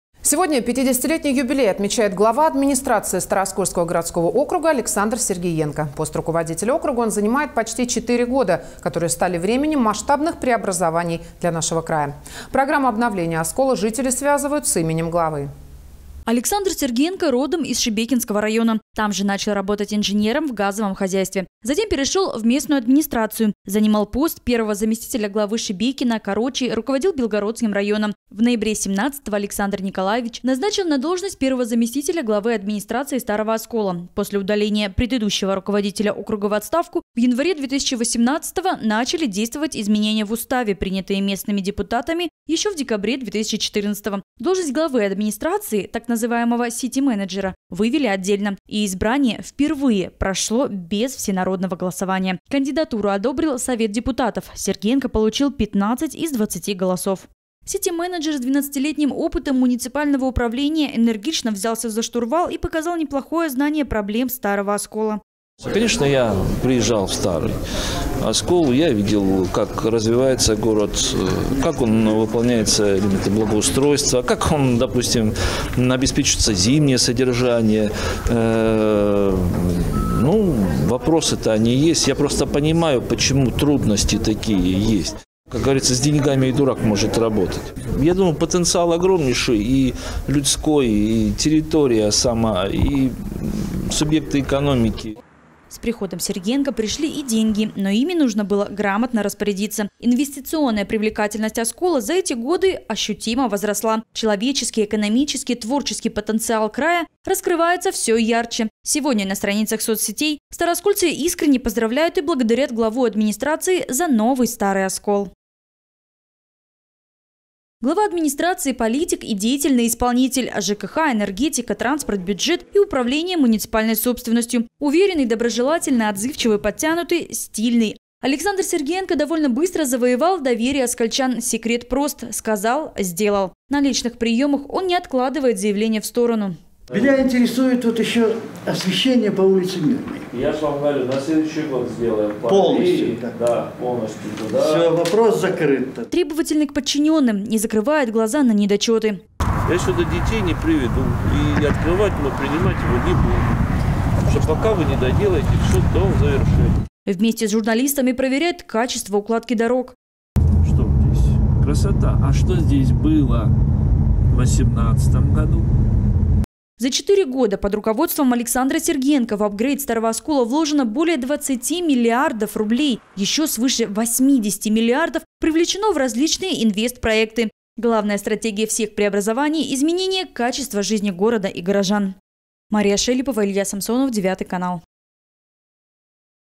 Сегодня 50-летний юбилей отмечает глава администрации Старооскольского горокруга Александр Сергиенко
Пост руководителя округа он занимает почти четыре года, которые стали временем масштабных преобразований нашего края. Программу обновления Оскола жители связывают с именем главы.